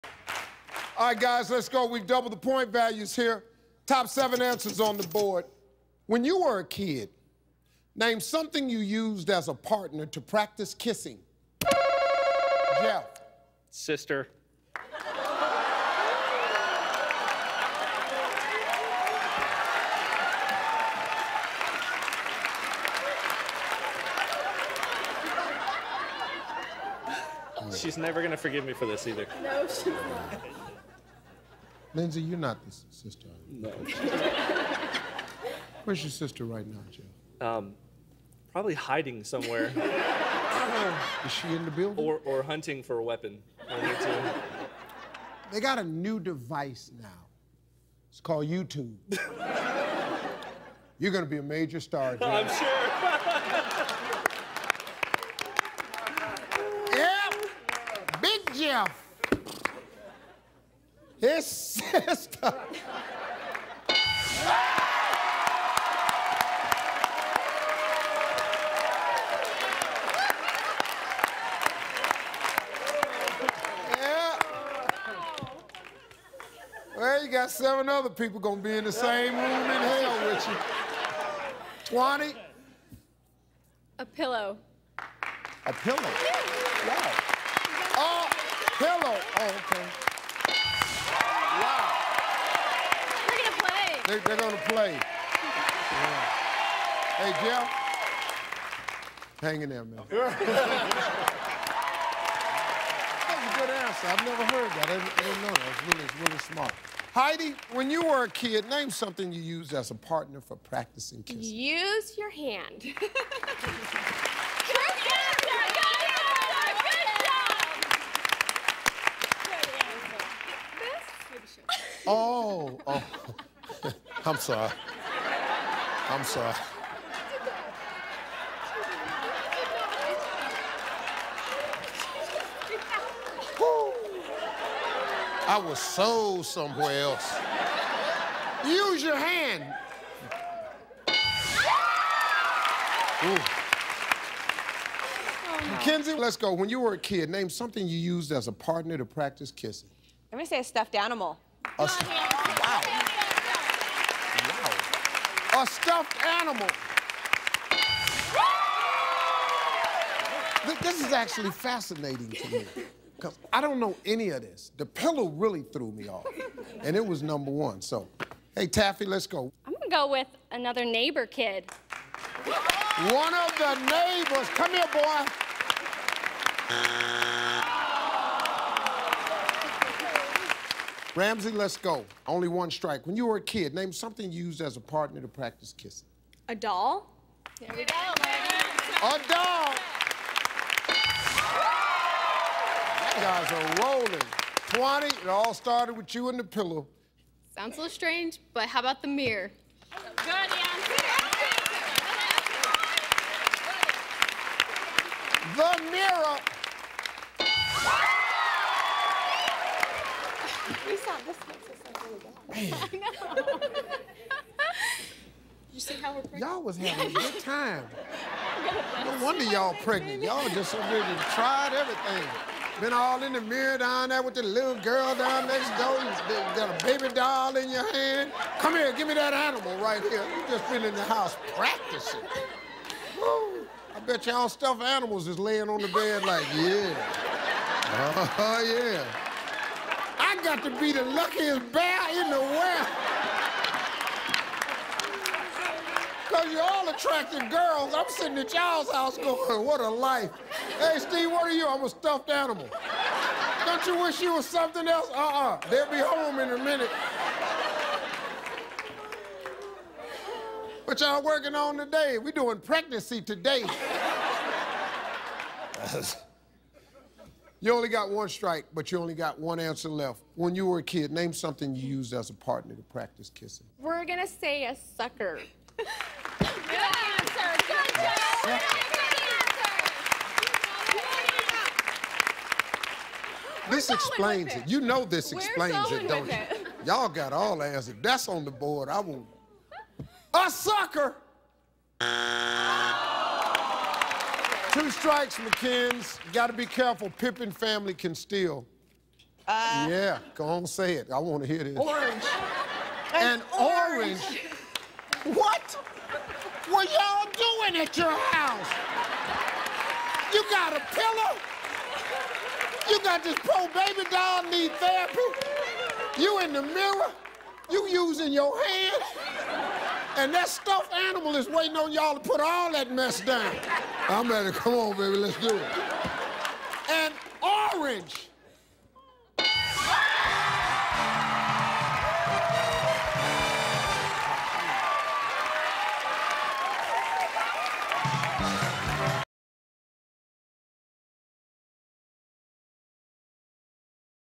You Kissed Your What?
When you were a kid, name something you used as a partner to practice kissing...